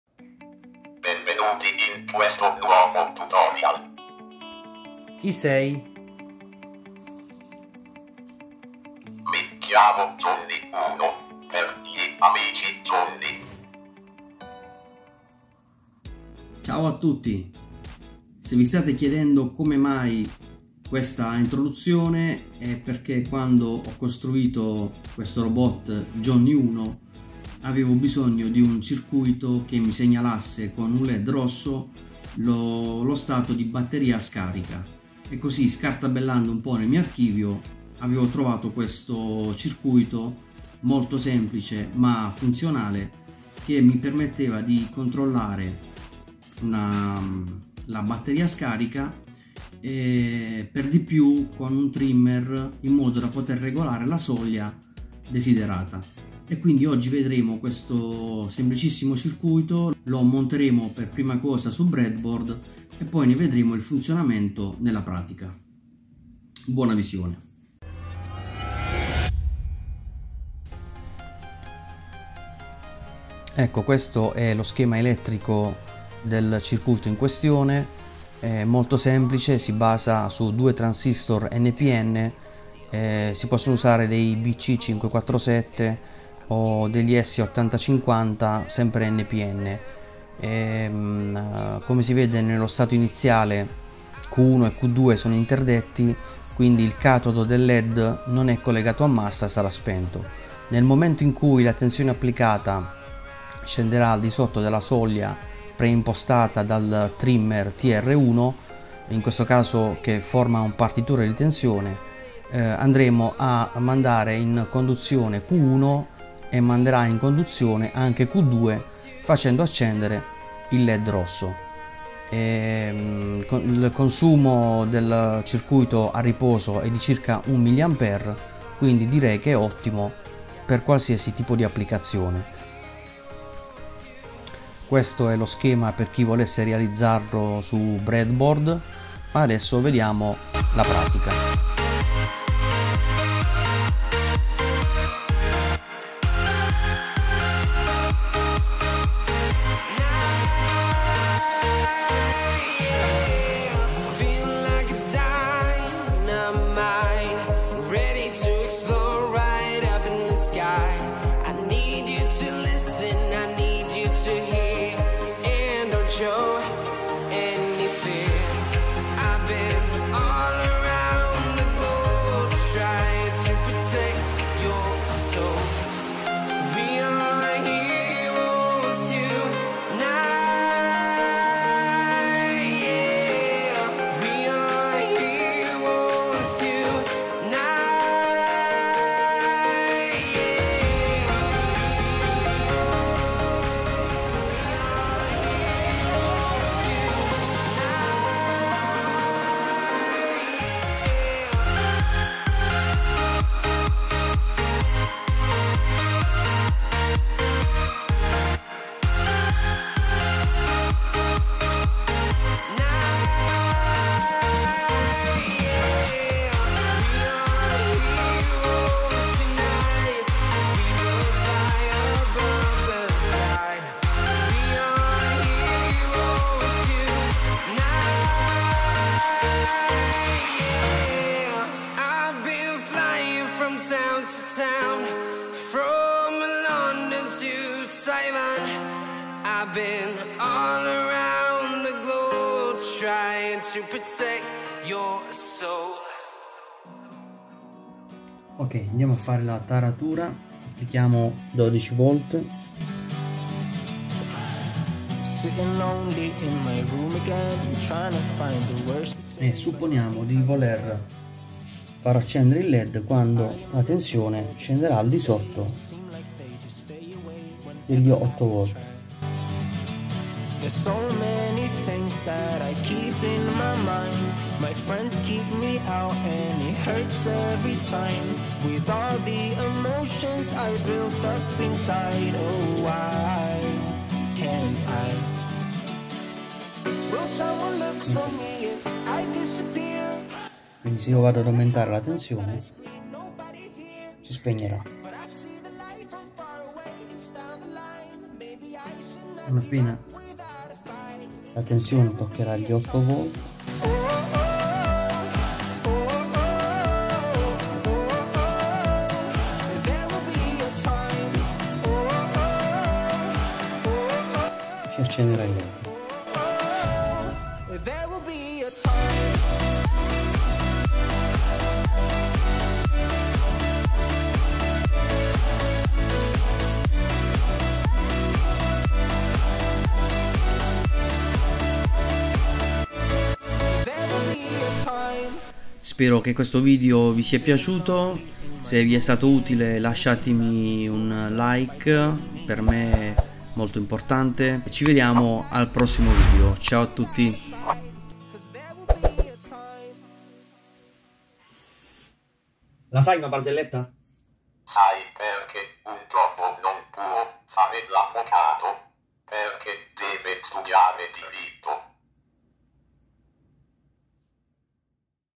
Spia batteria scarica - Circuito regolabile da 2 a 12 Volt
Un circuito semplicissimo che ci segnala lo stato di batteria scarica, con una soglia di tensione preimpostabile con un trimmer.
Lavora con una tensione di ingresso da 2 a 12 volt.

Capitoli:
0:00 Presentazione
1:10 schema elettrico
2:10 montaggio su breadboard
4:06 taratura e test del circuito
5:35 finale e barzelletta :)

Link per acquistare kit breadboard e componenti base

Per altre info e contatti usate pure il tasto 'Informazioni' qui sotto.